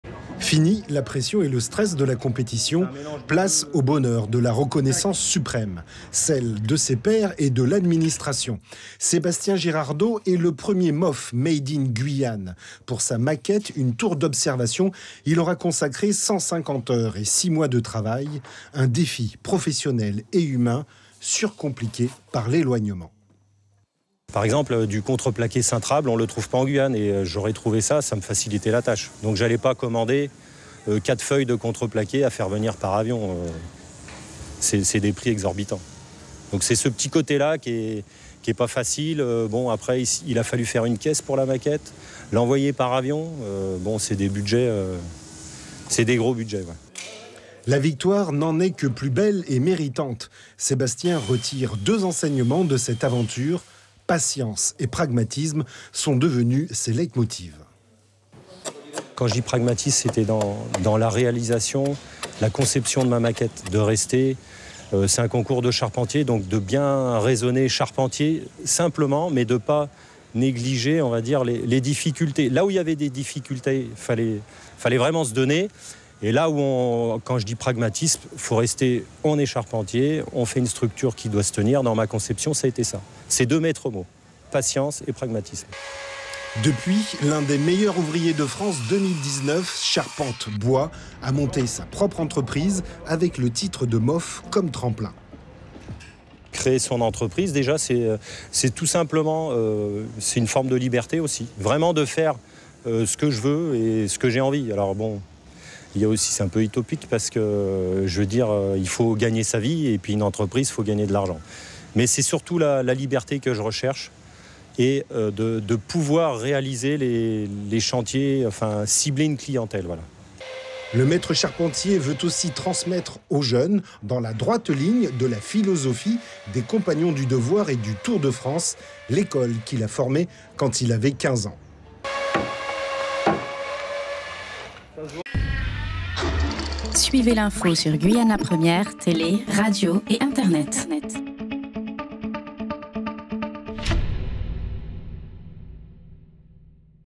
Sébastien Girardeau, lauréat du concours meilleur ouvrier de France dans la catégorie
Sébastien Girardeau, lauréat du concours meilleur ouvrier de France dans la catégorie charpente bois